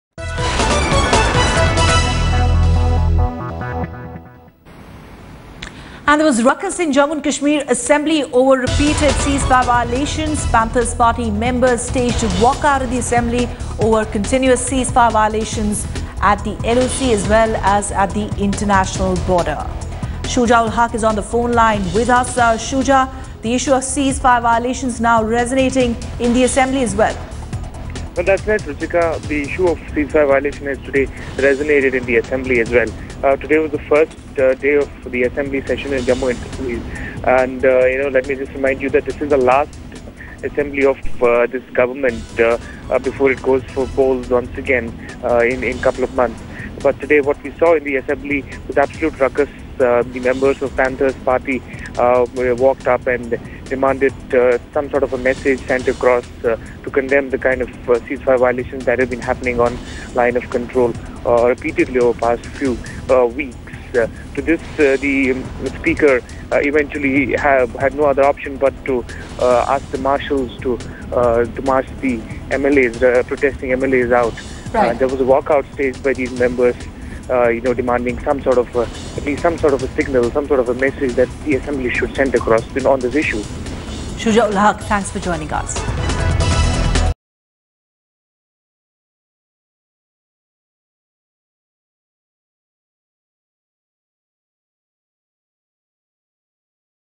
J&K Assembly: Panthers Party walks out of Assembly over ceasefire violations
The continuous attempts by Pakistan to violate the ceasefire between India and Pakistan has the J&K Panthers party highly miffed, especially seeing that the government is not taking any action to hit back Pakistan with a strong force. Listen in.